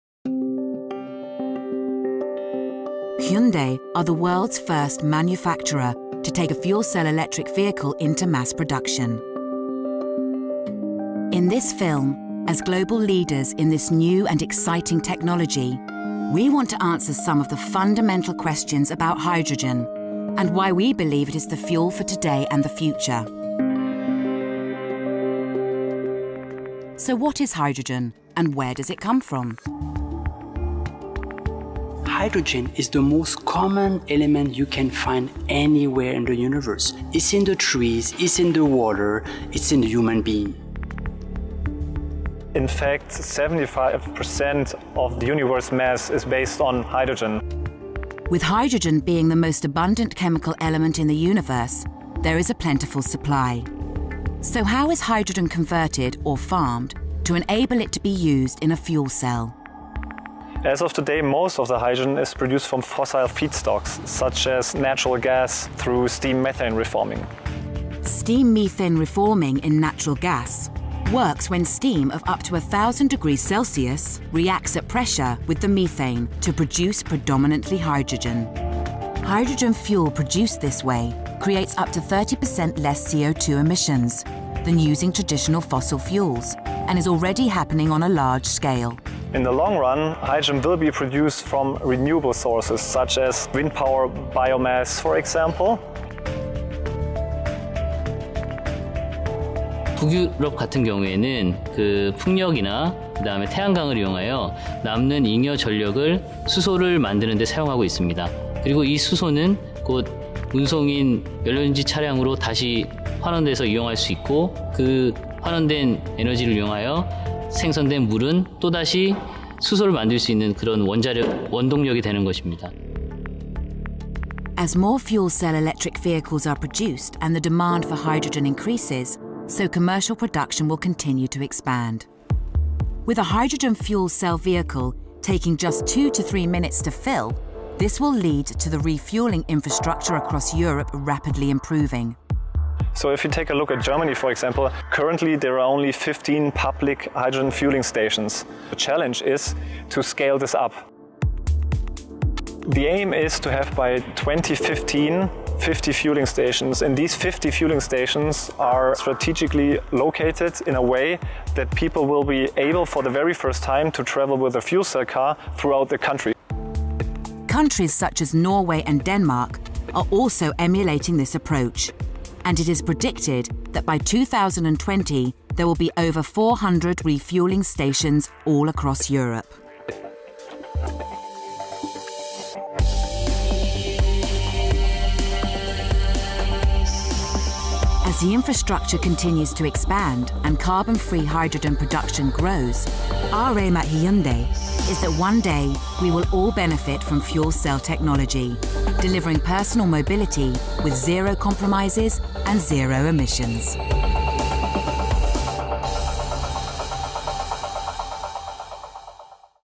Hyundai | ix35 | Fuel Cell | Introduction to Hydrogen Part 1
Hyundai is the first company to take a fuel cell electric vehicle into mass production. This film talks about some of the fundamental questions about hydrogen and why Hyundai believes it is the fuel for today and the future. Hydrogen is the most common element found on the planet. There is plenty of supply of Hydrogen. Most of the Hydrogen produced today is formed by steam-methane reforming. In the future, Hydrogen is expected to be produced by renewable sources such as wind power. Since Hydrogen is the fuel for the future and with the demand for fuel cell technology increasing the refueling infrastructure will rapidly improve. Countries such as Norway and Denmark are emulating this approach. It is expected that by 2020, there will be 400 refueling stations across Europe. Our aim at Hyundai is that all of us should benefit from fuel cell technology.